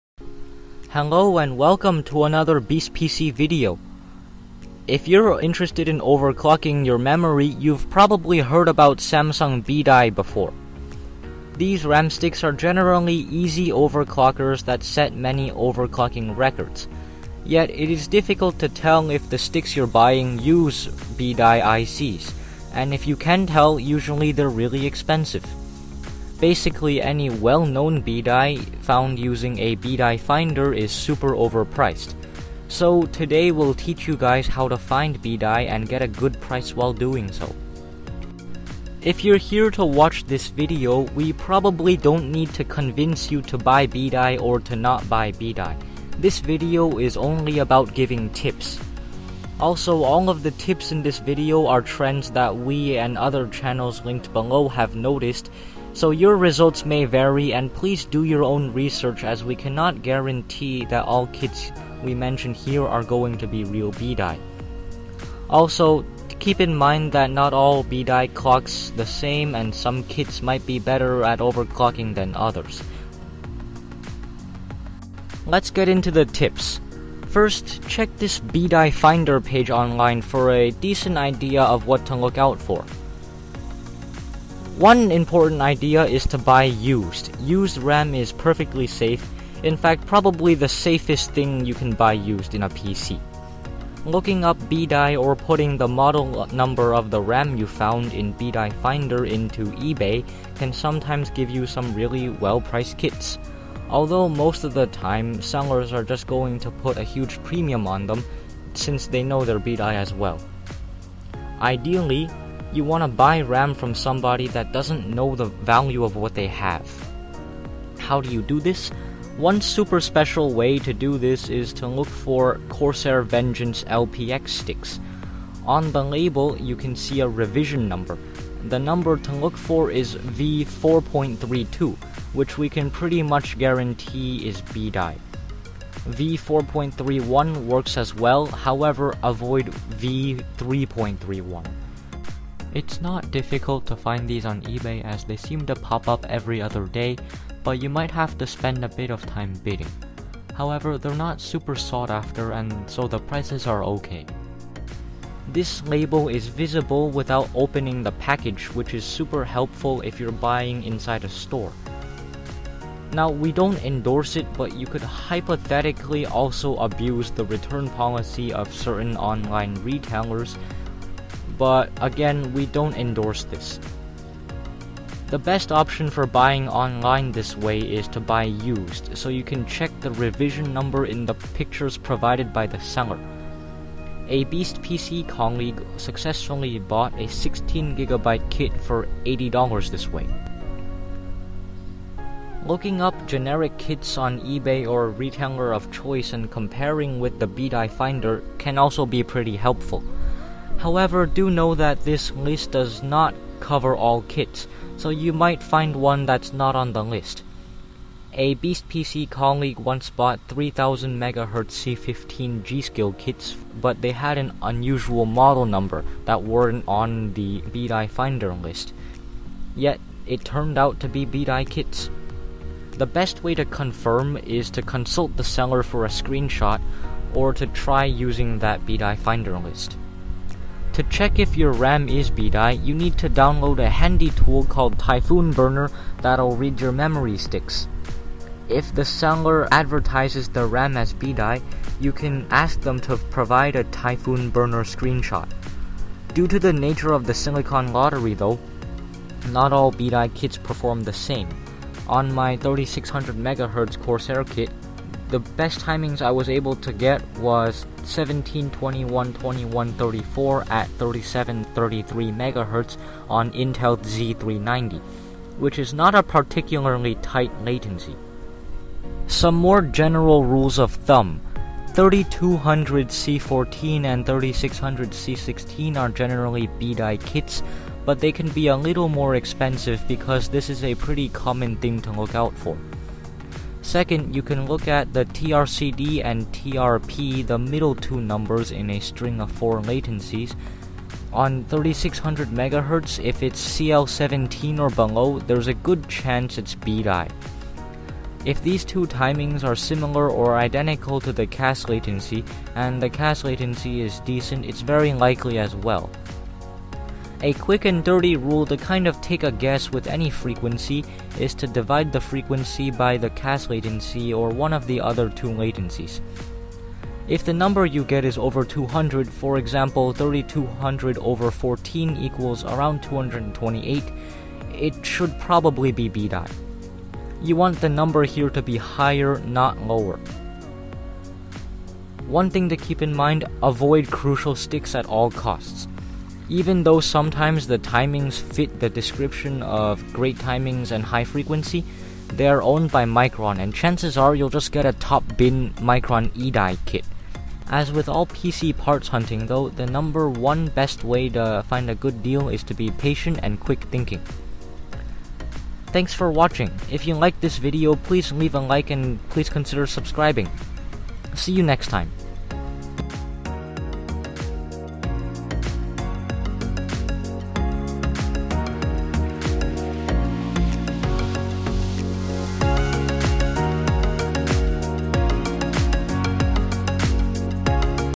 How to Get Samsung B Die RAM for Cheap
Purchasing Samsung B-Die is a great way to get fast, easy to overclock DDR4 RAM. But how can you do it for cheap? Today we find out and examine a few tips we’ve noticed. Keep in mind that not all you see in this video may be reproducible, and please do your own research prior to purchasing. We might be wrong. Also, please leave any special tips you’re willing to share in the comments below! Enjoy!

Model number mentioned in the video: CMK16GX4M2D3600C18

A very informative channel concerning memory and PC parts in general (Buildzoid at Actually Hardcore Overclocking)

Bitcoin: bc1qtrs6scl3yez0xuja62jtgmx22wk6fjaxvpzeag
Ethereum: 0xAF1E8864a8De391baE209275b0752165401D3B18